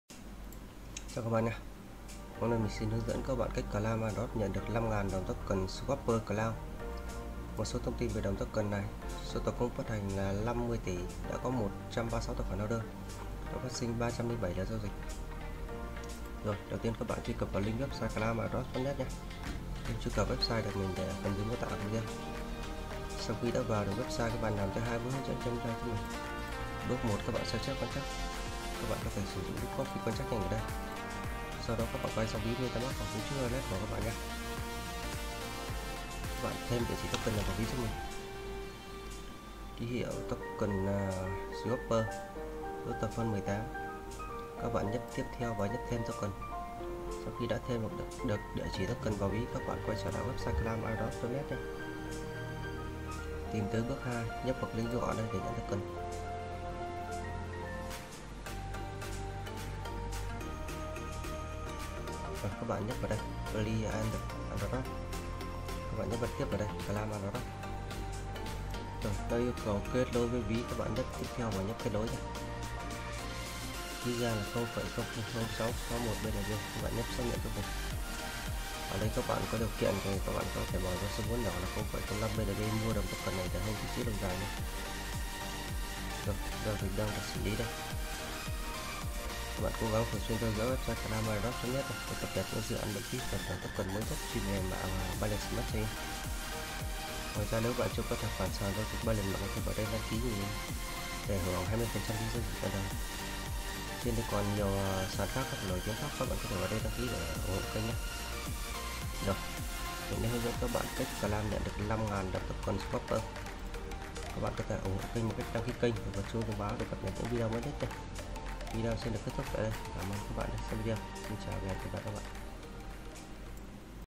Claim Airdrop Nhận 5,000 Token SWAPPER.CLOUD (SWAPPER), Mạng BSC Ví Trustwallet, MetaMask
+ Bước 1: Sao chép Contract :  0xb4255d056280d5c007331de6db2a2a01b1524d01

+ Bước 2: Truy Cập Web Tại Đây Click ( Claim Airdrop ) Nhận Token